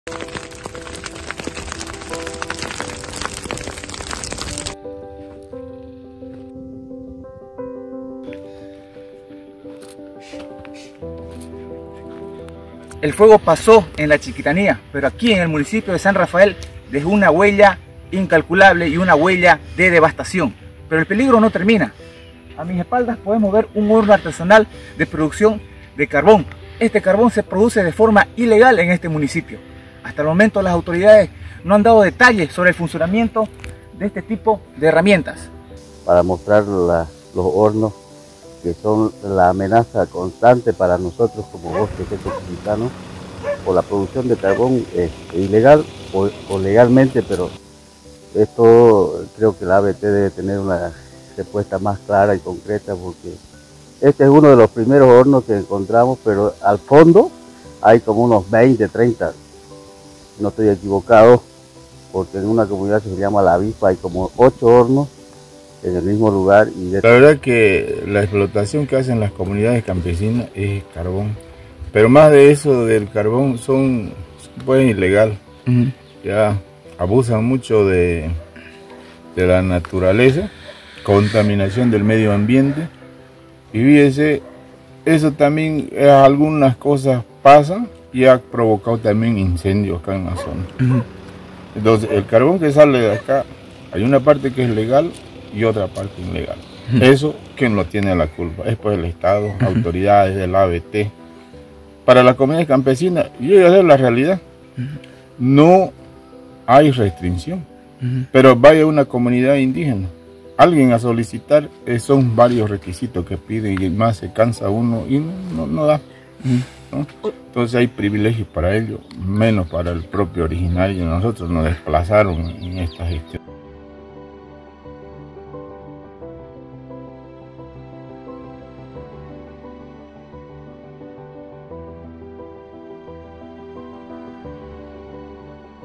🌳⚠️ Más de 500 hornos de carbón ponen en peligro el bosque de San Rafael
Maderas preciosas como el morado y el tajibo son taladas por las fábricas carboneras.